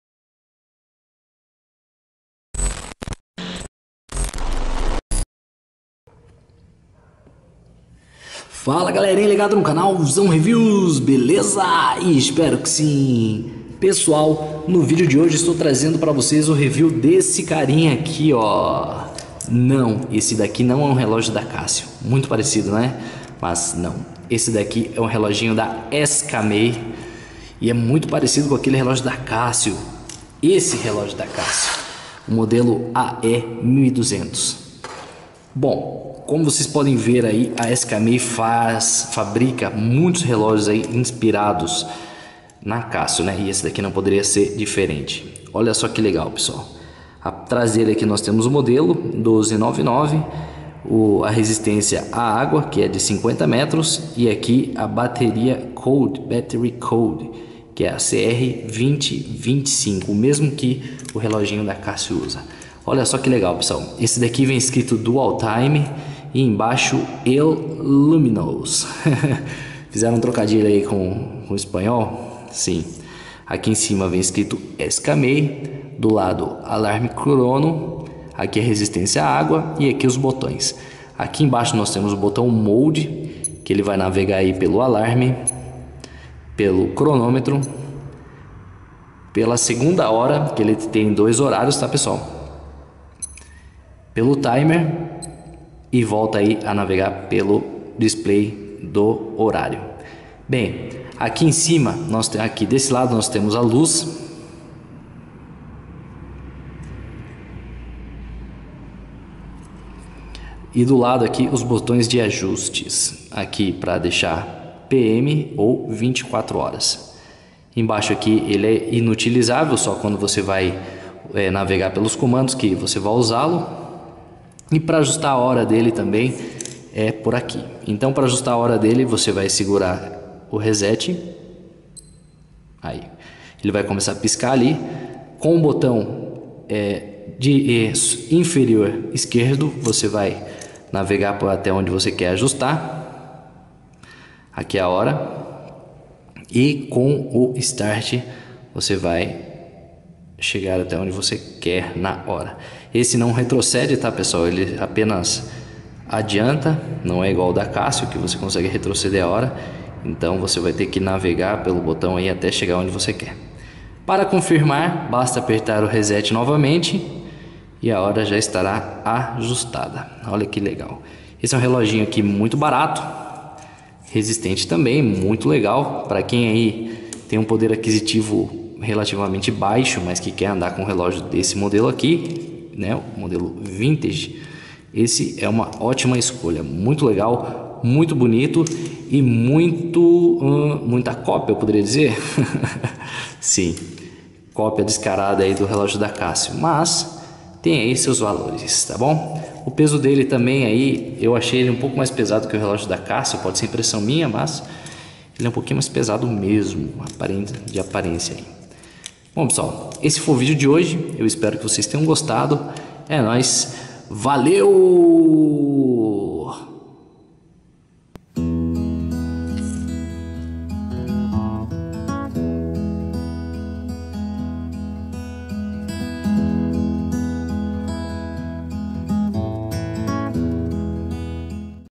Relógio Skmei 1299
Relógio skmei 1299, inspiração ou cópia descarada do relógio Casio ae-1200h-1v?
 Saiba que a Skmei tem uma infinidade de relógios "inspirados" nos lendários Casio G-Shock, f91w, casio ae1200, entre vários outros.

Os relógios da skmei, apesar da maioria (ou quase todos os relógios da marca) serem homages dos relógios Casio, são originais e com boa qualidade e acabamento (diferente das réplicas grotescas, que fazem da Casio por ai), então, se você não tem um alto poder aquisitivo, pode optar por algum dos modelos da skmei, e ainda ter total segurança e tranquilidade em comprar um relógio bom, bonito e barato!

Espero que você goste do vídeo, obrigado pelo seu like e é noiz!